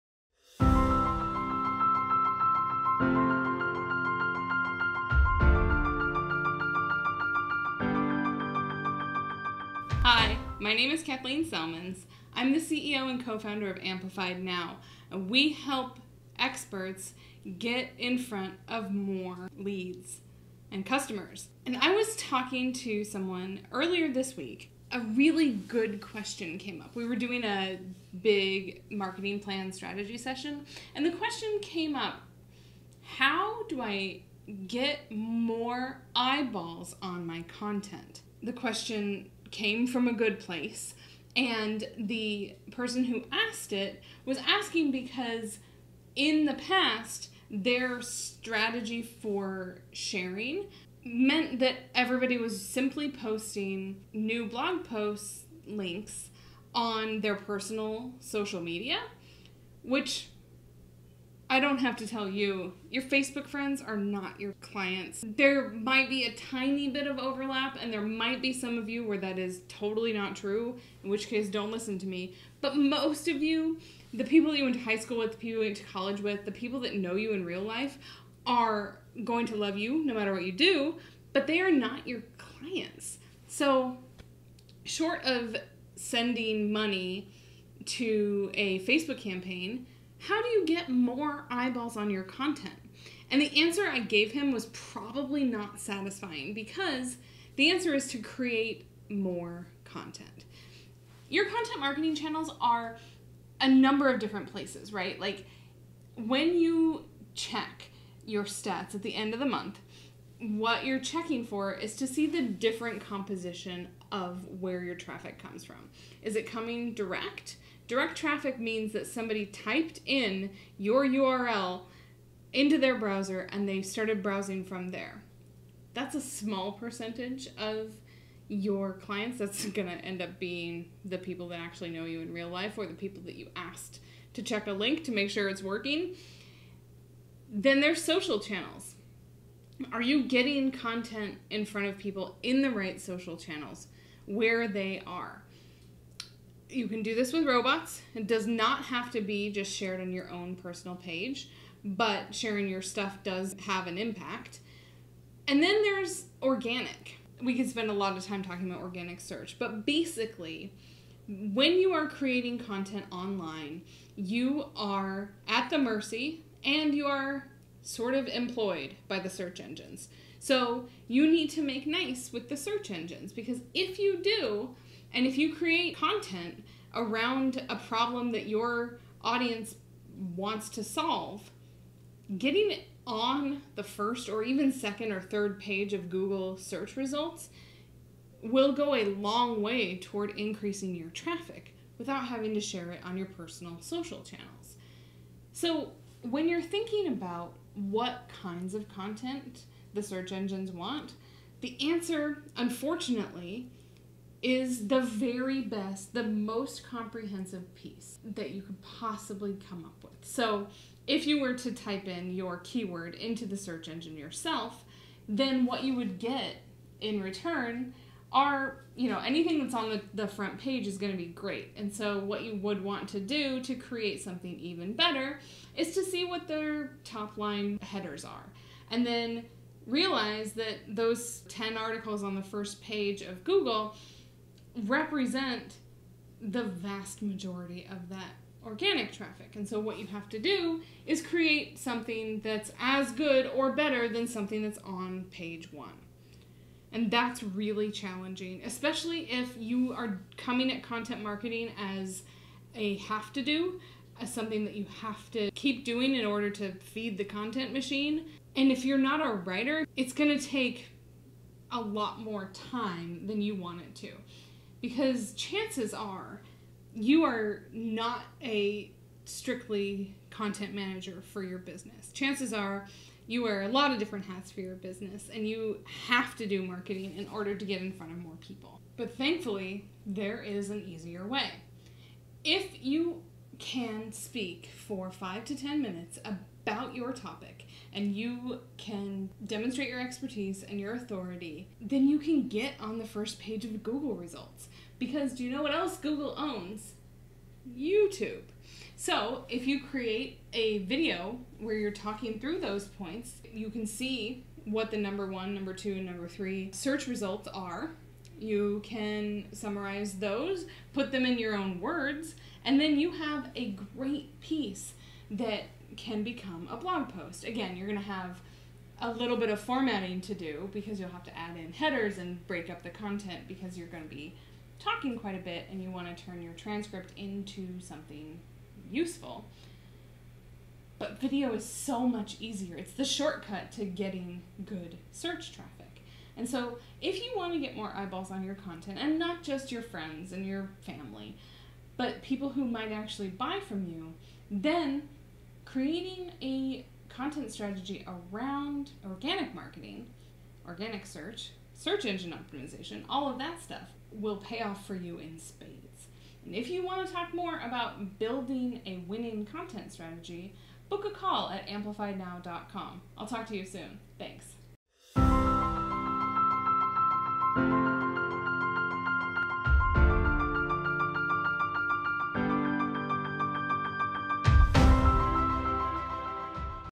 How to Get More Eyeballs on Your Content
Want to get more eyes on your content? Trust me, it often involves doing more than just sharing blog posts on social media or trying to compete against millions of other websites for SEO juice. Video can actually provide a great opportunity to get more eyes on your content and traffic back to your site. Here's why.